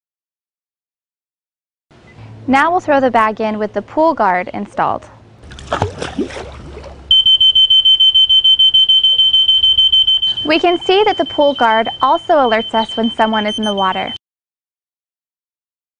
PoolGuard
Prueba del funcionamiento de la alarma para piscinas PoolGuard.